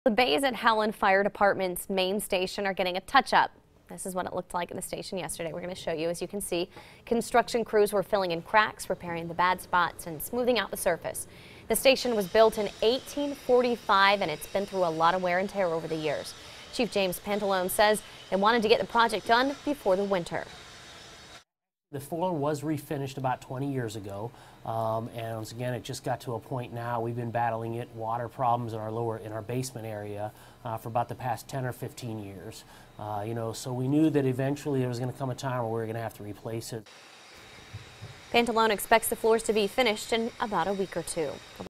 Howland Fire Department's main station undergoes floor repairs
On Wednesday, construction crews were filling in cracks, repairing any bad spots and smoothing out the surface.